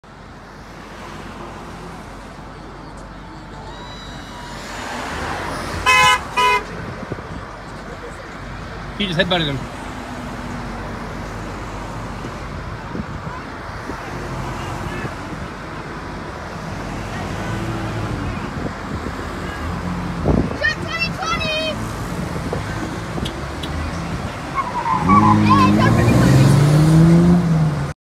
Trump supporter head butted by criminal
Kennewick Washington a trump supporter at a rally is head butted by a violent liberal